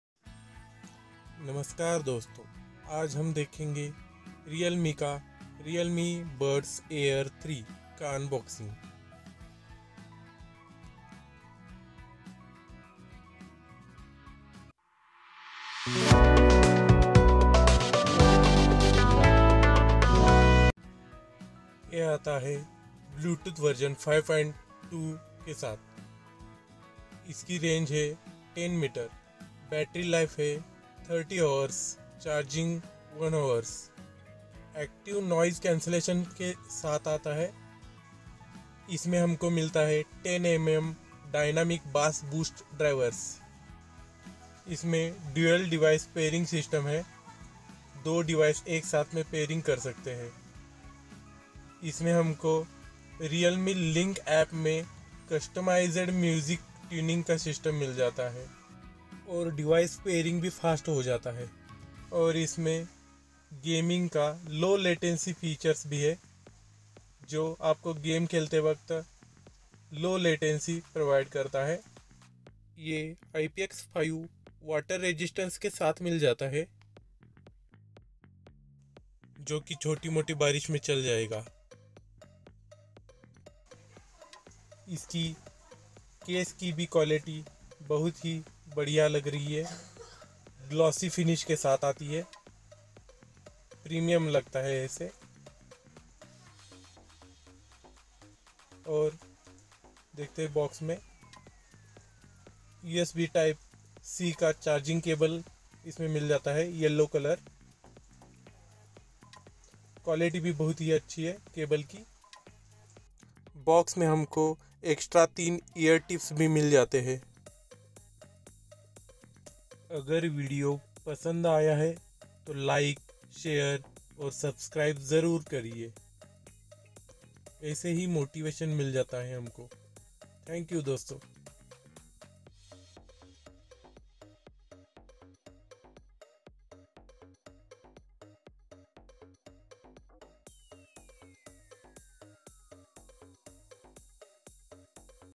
realme Buds Air 3 unboxing
realme Buds Air 3

• Wireless range: 10 m

Battery life: 30 hrs | Charging time: 1 hours

• Active Noise Cancellation (up to 42dB)

10mm Dynamic Bass Boost Drivers

Up to 30hrs Non-stop Playback | Charging Time - 1

hour • Fast Charging Supported - 10min charge for 100mins

playback

Dual Device Pairing | Smart Wear Detection with

Google Fast Pair

• Customized Music Tuning & Dolby Audio (on supported devices)

IPX5 Water Resistant Super Low Latency Gaming Mode

realme Link App Connectivity